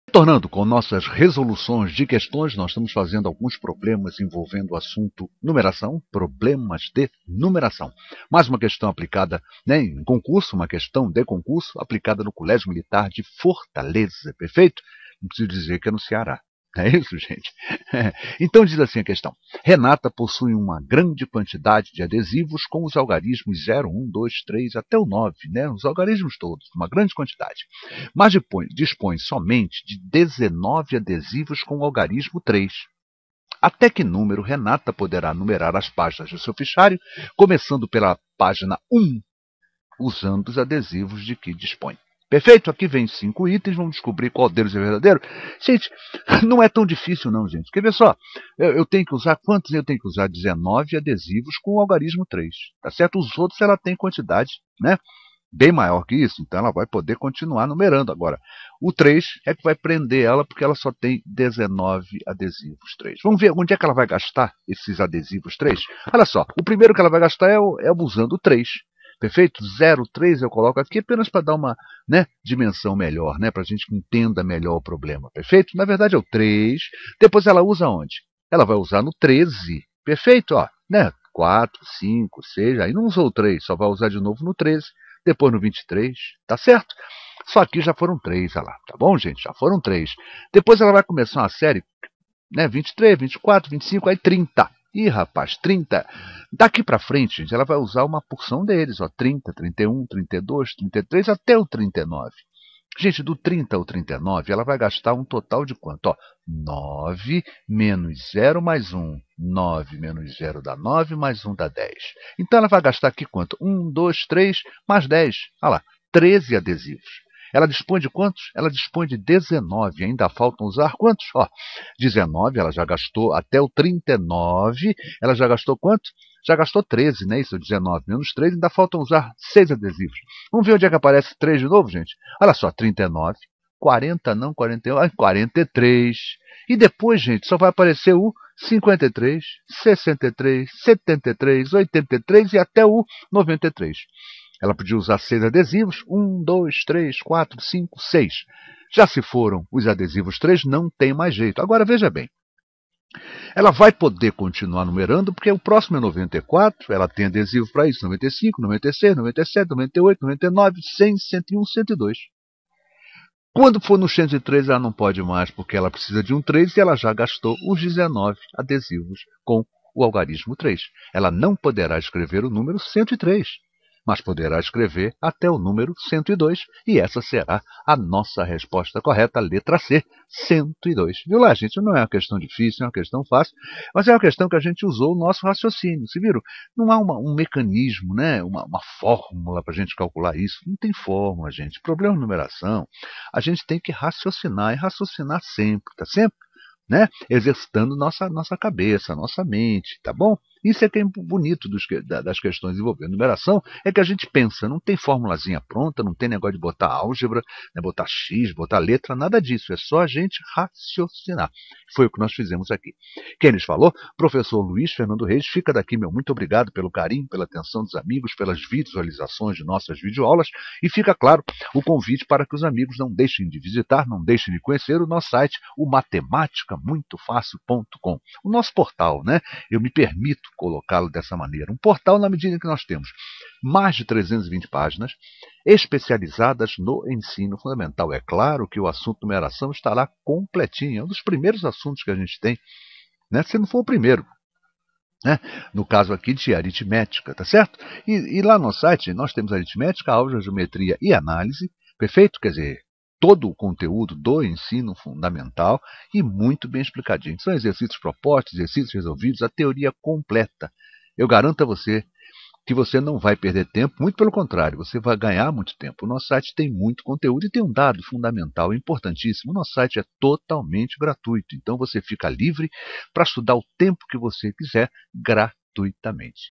# Colégio Militar de Fortaleza Numeração Questão 109 CD
Colégio Militar de Fortaleza   Numeração   Questão 109 - Videoaula Explicada pelo professor Luiz Fernando Reis
Vídeo educacional e livre para qualquer idade.Videoaula Explicada